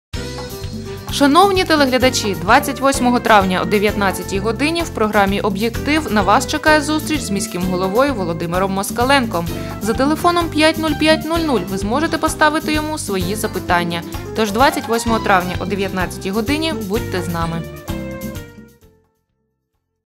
КоростеньТВ_22-05-15_Внимание! Прямой эфир!
Увага! 28 травня о 19:00 годині зустріч в прямому ефірі з міським головою В. Москаленком.